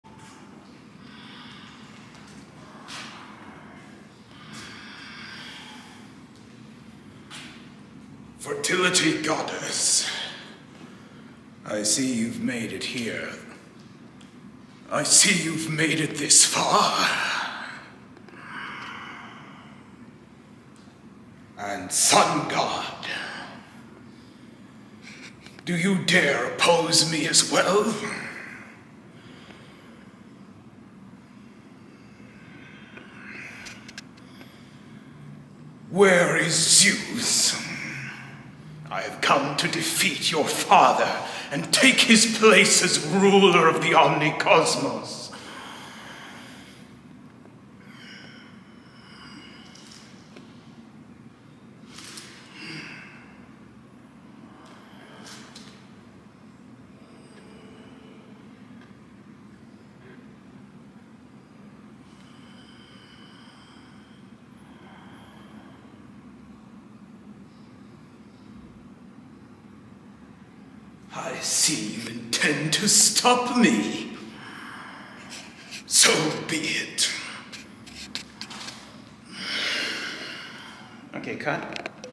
Demeter and Typhon the Gods attack C0933
Typhon a being that is born of Gia the primordial immortal of earth and Trtarus the being that sustains and is a horrible deep place at the lowest depths of Hades is met by the Titan of the harvest Kronos recently released from Tartarus by Eris Goddess of discord and Chaos in an attempt to over throw Zeus God of fate, law, order, the heavens and power approaches Apollo God of the sun, truth, light, medicine, music, justice, plagues, beauty, self restraint, archers and oracles.